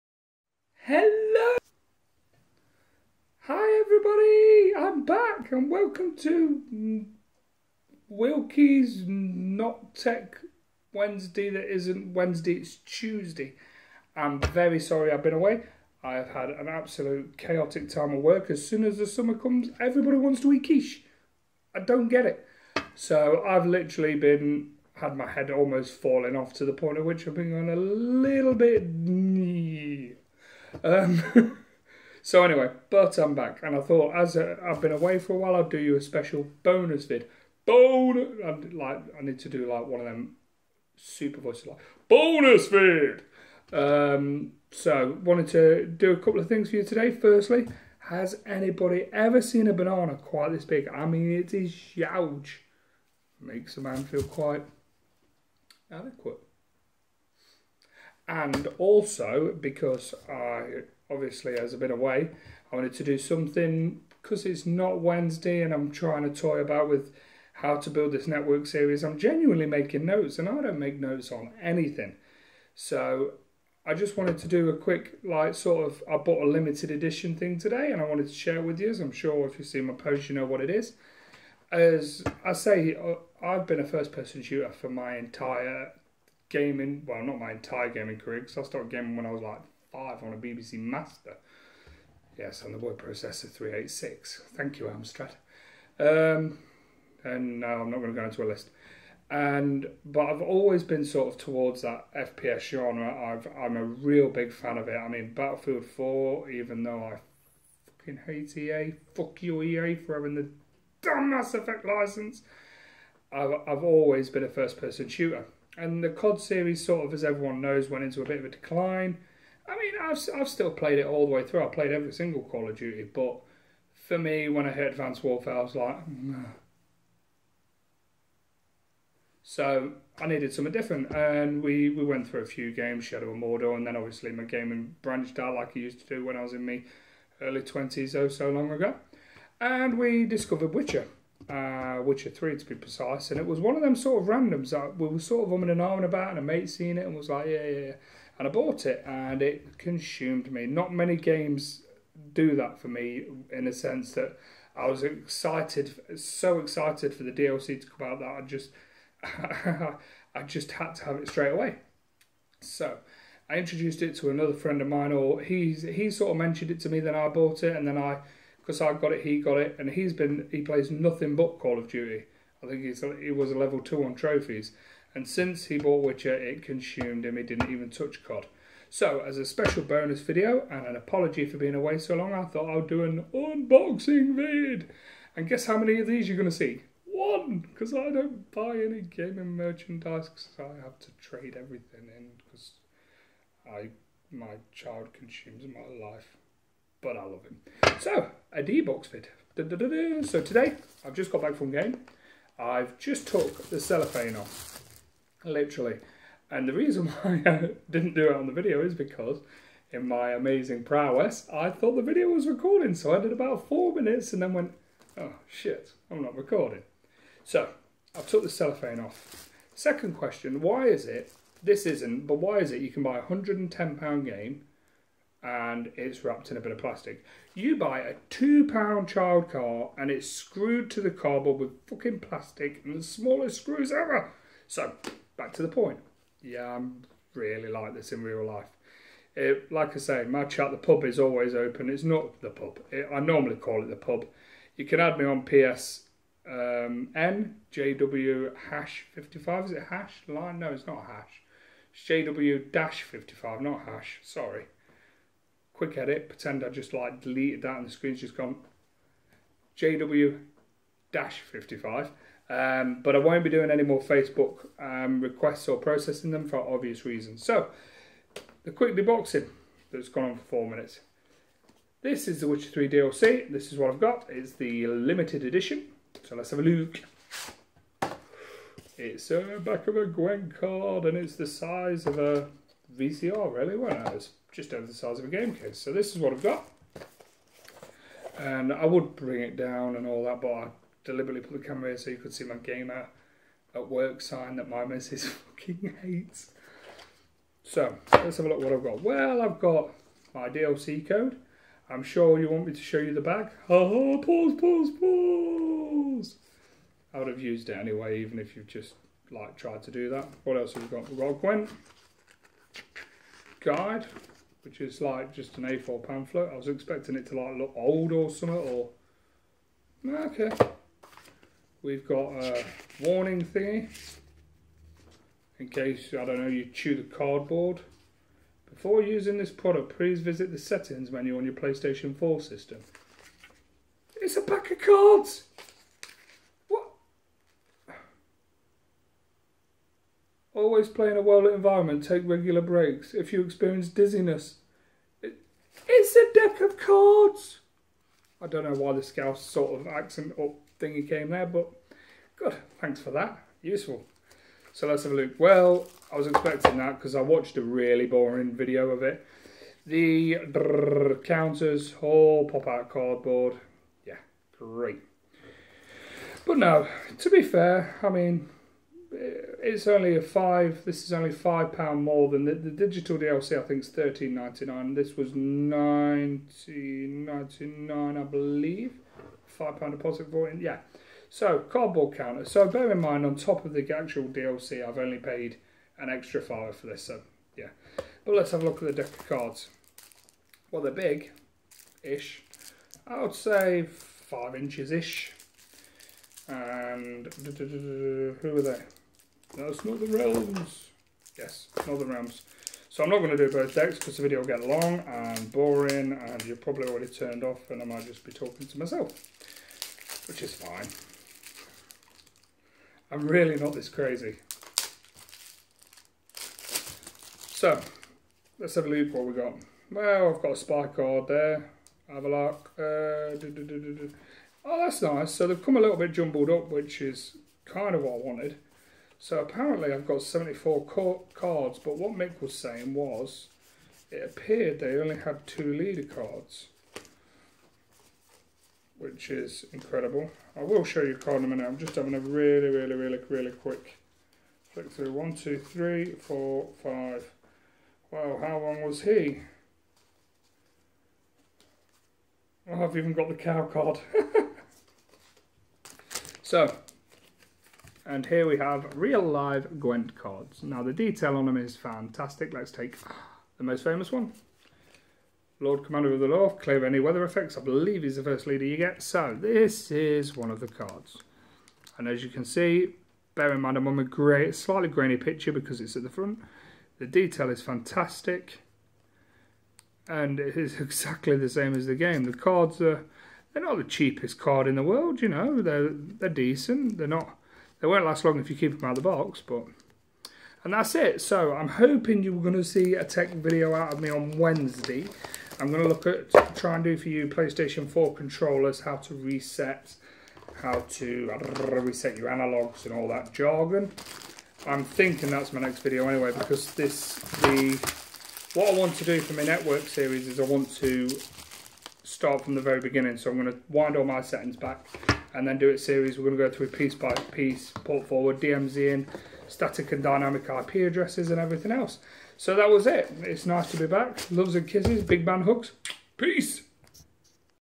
Witcher 3 limited edition blood and wine unboxing and having a giggle
This Video was just some fun to make a long time ago.  Eventually financial constraints meant I had to sell them but what I wouldnt give to get them back.